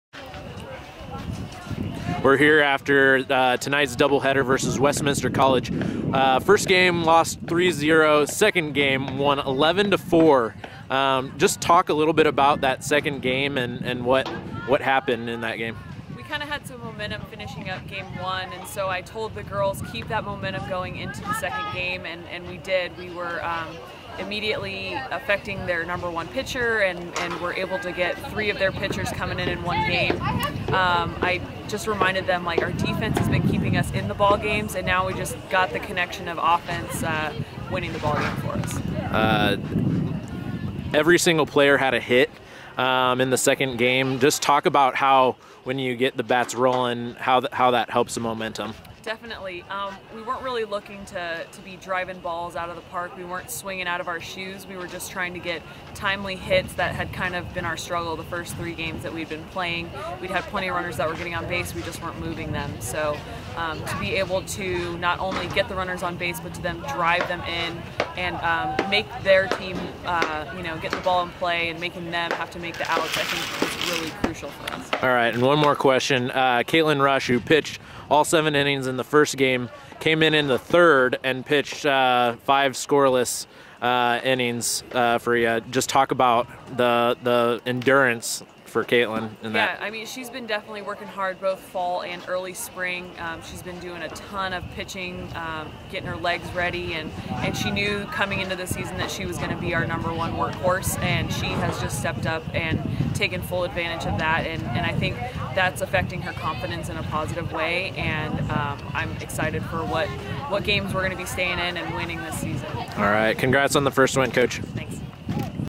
Postgame Praise w/ SB Coach Dean (3/11/15)
Stephens College softball earned its first victory of 2015 on Wednesday, splitting with Westminster College. Stars' head coach Tracy Dean breaks down the 11-4 victory in Game 2.